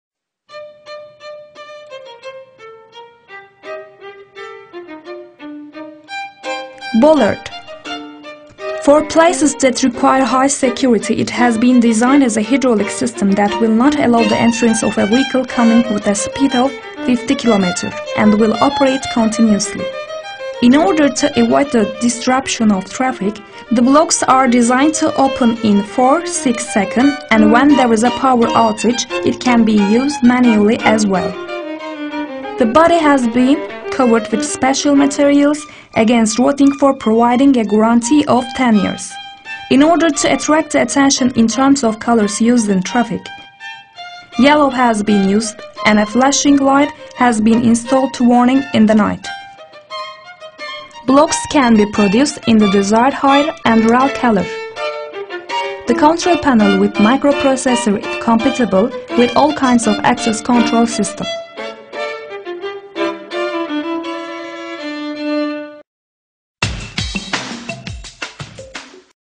Hidrolik mantar bariyer - Hydraulic bollard
Bollard For places that require high security, it has been designed as a hydraulic system that will not allow the entrance of a vehicle coming with a speed of 50 km. and will operate continuously. Hydraulic bollard In order to avoid the disruption of traffic, the blocks are designed to open in 4-6 sec. and when there is a power outage, it can be used manually as well. Rising Bollard The body has been covered with special materials against rotting for providing a guarantee of 10 years. In order to attract the attention in terms of colors used in traffic, yellow has been used and a flashing light has been installed to warning in the night. Blocks can be produced in the desired height and ral color. Bollard The control panel with microprocessoris compatible with all kinds of access control system.